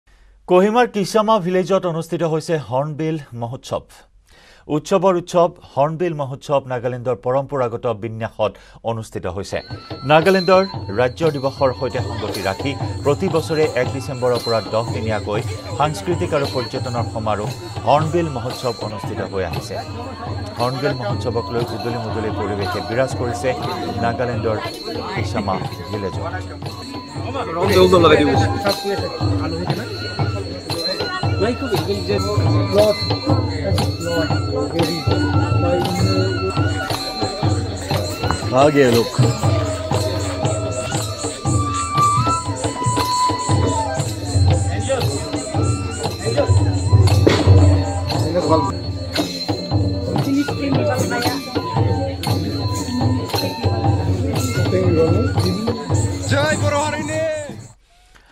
Hornbill Festival | Kohimaৰ কিছামা ভিলেজত হৰ্ণবিল ফেষ্টিভেলৰ আয়োজন
১০ ডিচেম্বৰলৈ চলিব এই ফেষ্টিভেল । Nagalandৰ ৰাজ্যিত দিৱসৰ লগত সংগতি ৰাখি Hornbill Festivalৰ আয়োজন ।